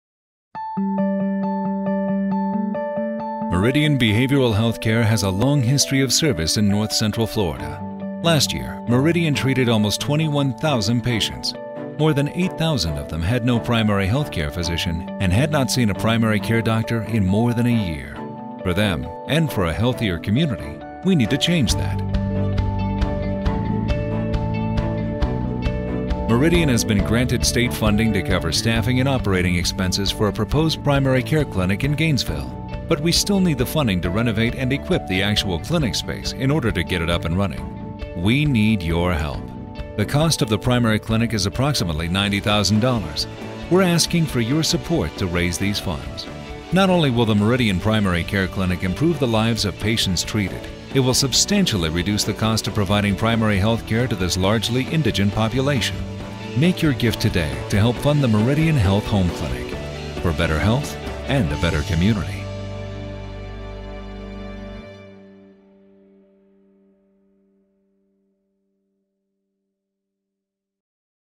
MeridianPrimaryClinic HD
Putting a roof over healthcare- Creating a health home for the most vulnerable
Last year, Meridian Behavioral Healthcare treated almost 21,000 patients in North Central Florida. More than 8,300 of those patients (approximately 40%) have no primary health care physician and have not seen a primary care doctor in a year or more.

Individuals with severe mental illnesses and substance use disorders are particularly likely to have co-morbid illnesses that are also chronic in nature, noted Dr. Maggie Labarta, president and CEO of Meridian.

We want to meet the needs of those who have no primary care physician; Meridian has been granted state funding to cover the staffing and operating expenses for a proposed Health Home Clinic in Gainesville.

We are excited about the operational funding the state will provide, but we still have to provide funding to renovate and equip the actual clinic. To do that, we need your help!

The cost of the renovating and equipping the 1,200 sq. ft. Health Home Clinic is approximately $90,000 and we are seeking your support to help us raise these funds.

Here is when you come in, one of the major expenses of creating this clinic will be equipping it. So we are reaching out to the community for help.

It is our hope to raise the money to equip at least one of the three clinic rooms via this crowd funding campaign. The cost of equipping one room is 6,000+ dollars.

The need for this primary care clinic is great and Meridian is determined to make this dream a reality.

As a community we can help provide primary care to individuals in dire need. The long-term effect of this project will result in a healthier more efficient community as a whole. Meridian feels it is our responsibility to provide the best and most comprehensive care possible to our clients. This will take not only the efforts of our dedicated staff but the compassion and dedication of our community to make this happen.